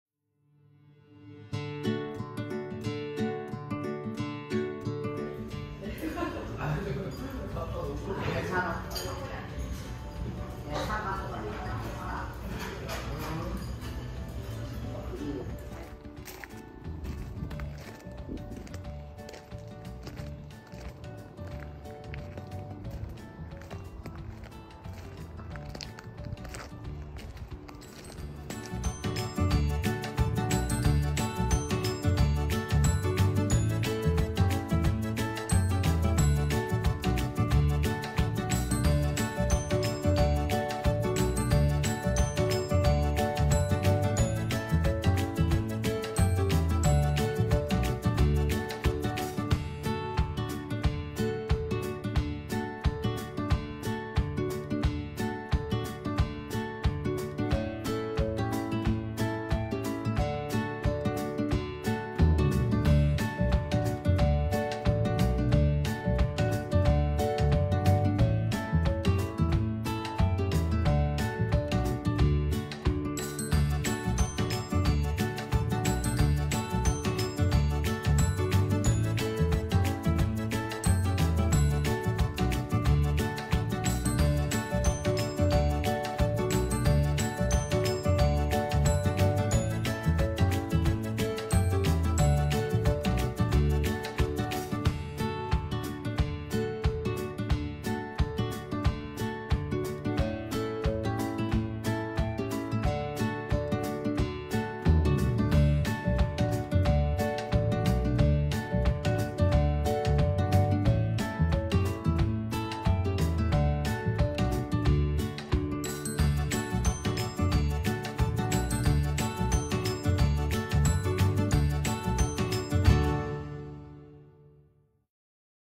[보들라이프] 김해 장어덮밥 맛집 바자라 (feat. 정원뷰 최고!!)
김해쪽 갈일이 있으면 생각나는 맛집인 김해 바자라에 다녀왔습니다!!
날씨가 풀릴수록 가게 앞 정원이 더 멋져지는것 같습니다~
직원분도 상당히 친절하고 무엇보다 장어덮밥이 너무 맛있습니다~
정갈한 차림새도 좋고 은근히 양도 많아요!!
김해가면 항상 들리고 싶은 맛집입니다^^